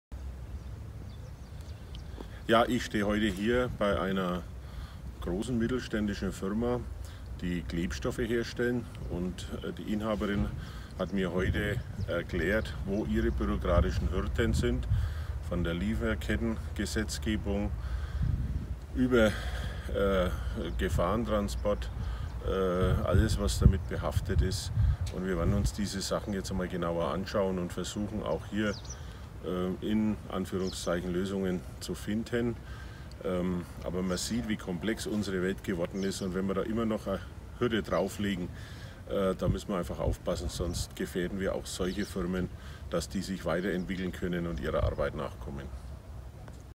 Lieferkettengesetz in der Kritik - Bayern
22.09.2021: Im Gespräch mit MdL Walter Nussel kritisiert DELO-Chefin Sabine Herold bürokratische Vorgaben wie das Lieferkettengesetz, die die Zukunft mittelständischer Unternehmen gefährden.